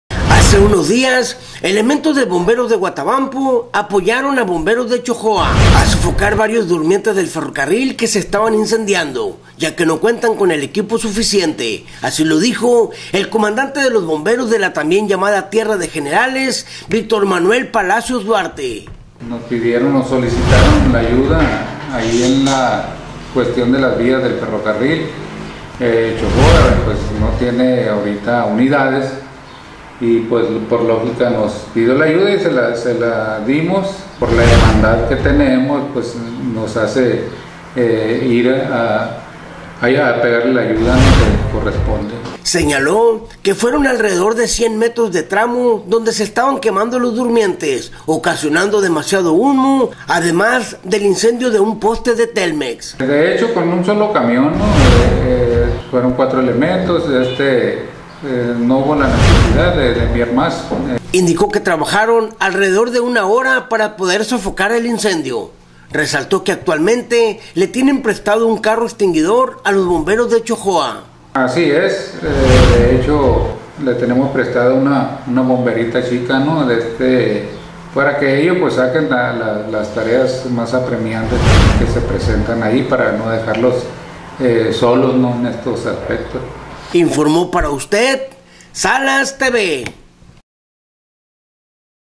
¡¡¡BOMBEROS DE HUATABAMPO APOYAN A BOMBEROS DE ETCHOJOA!!!
REPORTERO Y CAMAROGRAFO: MANUEL SALAS
28 MAR 2022
Hace unos días elementos de bomberos de Huatabampo apoyaron a bomberos de Etchojoa a sofocar varios durmientes del ferrocarril que se estaban  incendiando ya que no cuentan con el equipo suficiente así lo dijo el comandante de los bomberos de la también llamada tierra de generales Víctor Manuel Palacios Duarte
“Nos solicitaron la ayuda ahí en la cuestión de las vías del ferrocarril de Etchojoa pues no tiene ahorita unidades y pues por lógica nos pidió la ayuda y se la dimos, por la hermandad que tenemos pues nos hace ir a pegarles la ayuda que corresponde”
Señalo que fueron alrededor de 100 metros de tramo donde se estaban quemando los durmientes, ocasionado demasiado humo además de un poste del Telmex
“De hecho con un solo camión fueron 4 elementos y no hubo la necesidad de enviar más”
Indicó que trabajaron alrededor de una hora para poder sofocar todos los durmientes y el poste
Resaltó que actualmente le tienen prestado un carro extinguidor a los bomberos a Etchojoa
“Así es de hecho le tenemos prestada una bomberita chica para que ellos saquen las tareas más apremiantes también que se presentan ahí para no dejarlos solos en estos aspectos”
Informó para usted, SALAS-TV….